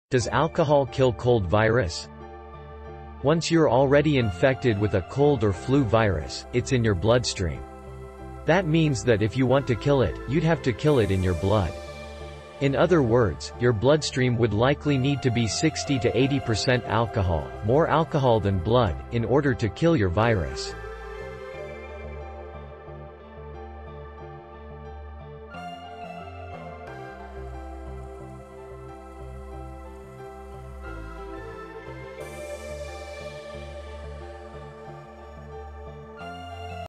Does alcohol kill cold virus?
Once you’re already infected with a cold or flu virus, it’s in your bloodstream. That means that if you want to kill it, you’d have to kill it in your blood. ... In other words, your bloodstream would likely need to be 60 to 80 percent alcohol (more alcohol than blood!) in order to kill your virus.